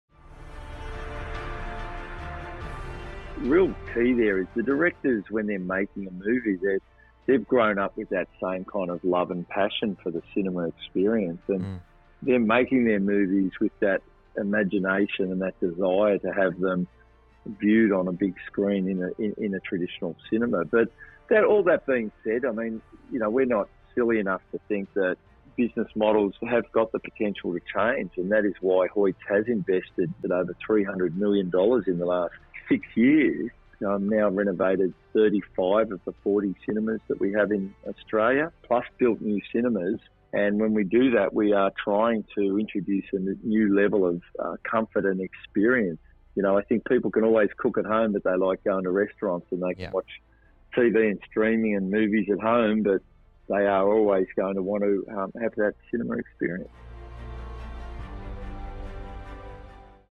Hoyts CEO Damian Keogh Audiogram
With cinemas reopened across most of Australia (Melbourne, we see you and are thinking of you), it felt like a good time to share this snippet from our chat with Hoyts CEO Damian Keogh. We agree with Keogh that there's nothing like the shared cinema experience and a night out seeing a movie on the big screen.

To hear the rest of Popcorn Podcast's insightful interview with Damian Keogh, check out the full bite-sized episode